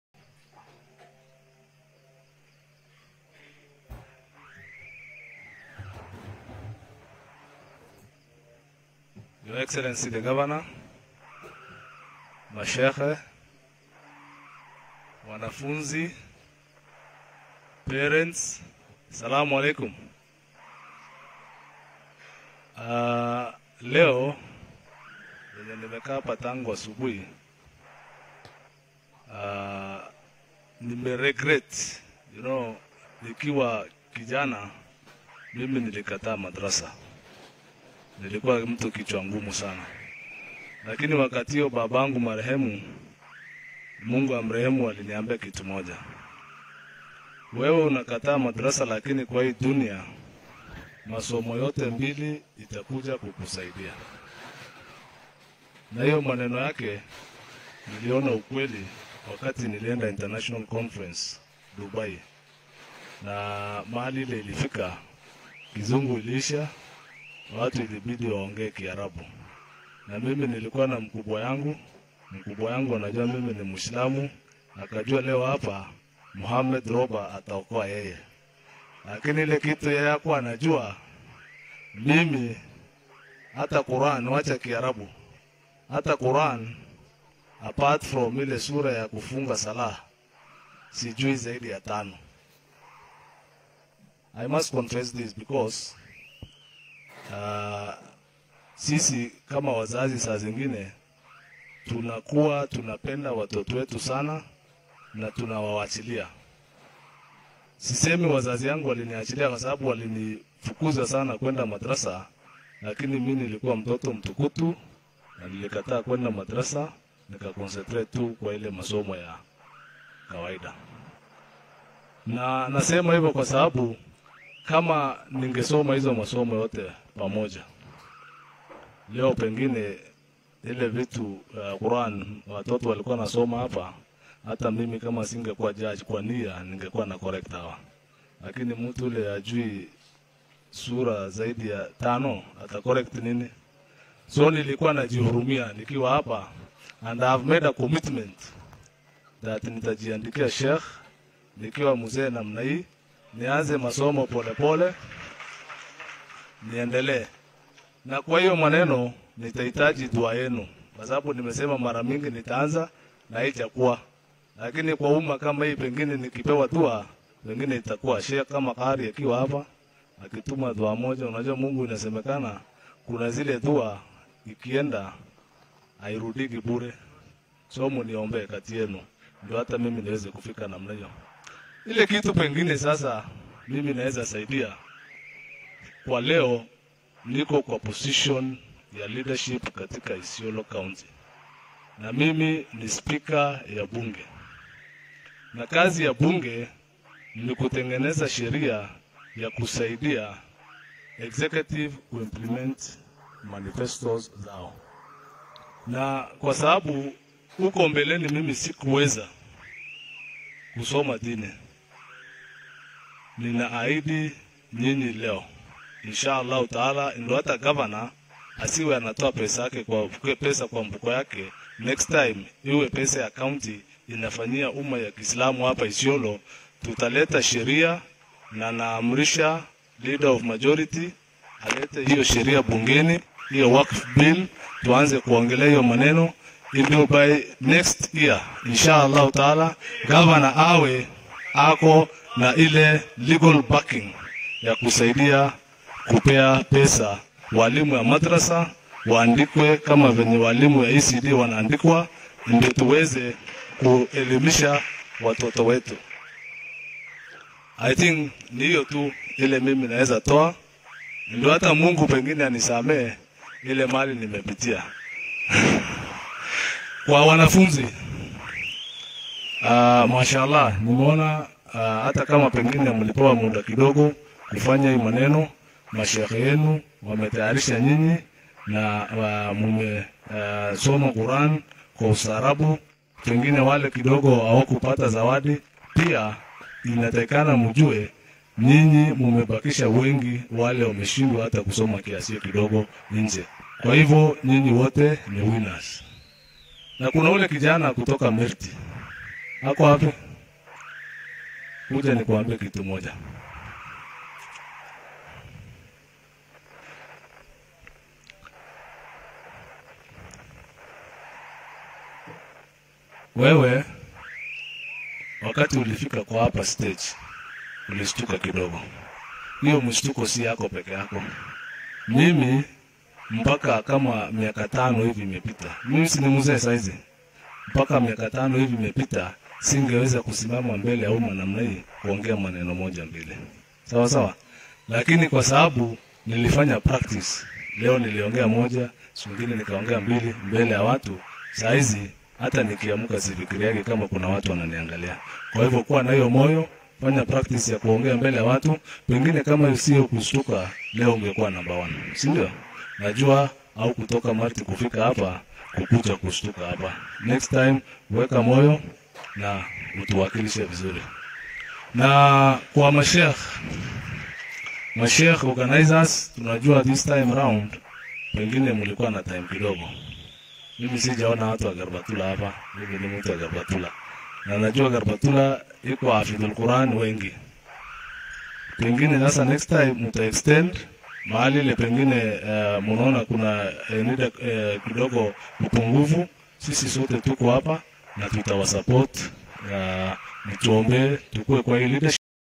Speaker Hon. Mohammed Roba OGW, CSMP (R), M.ISMI, gives his speech at the Quran competition forum
Hon. Mohammed Roba OGW, CSMP (R), M.ISMI, speaker of the  County Assembly of Isiolo, announced plans to introduce a bill supporting religious activities during a speech at the Quran competition forum